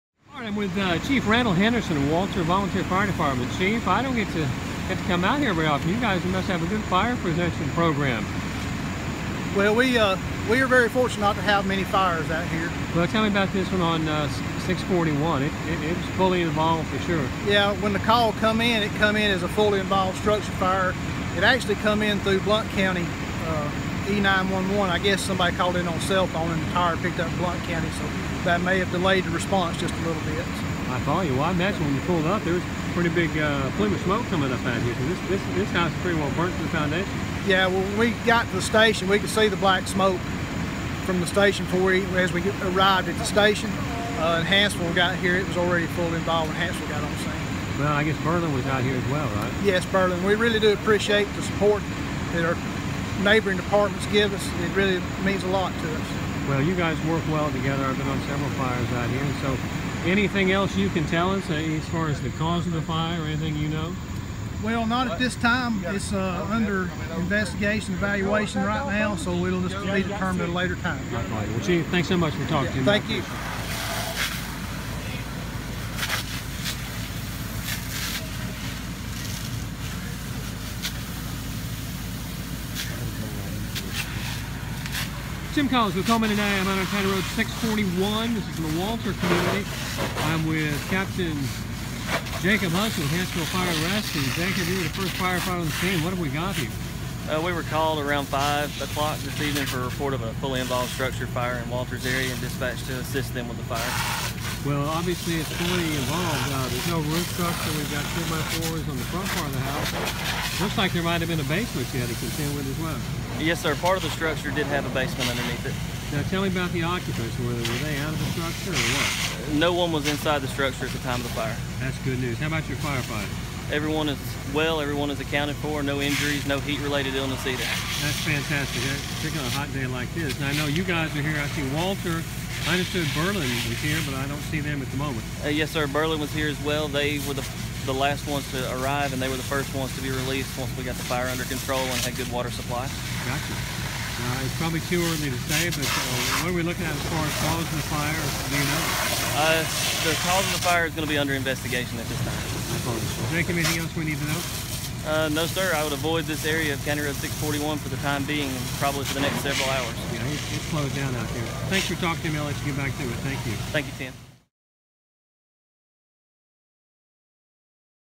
INTERVIEW: FIRE CHIEF RANDALL HENDERSON & CAPTAIN JACOB HUDSON
During tonight's house fire in the Center Hill community, we spoke with both Captain Jacob Hudson of Hanceville Fire & Rescue and Fire Chief Randall Henderson of Walter Fire Volunteer Fire Department.

Neither man was willing to speculate on the nature of the fire itself, or what may have caused it. They did provide some fascinating insights into the incident and how their respective departments responded in this interview.

For full details on this fire along with images, please see: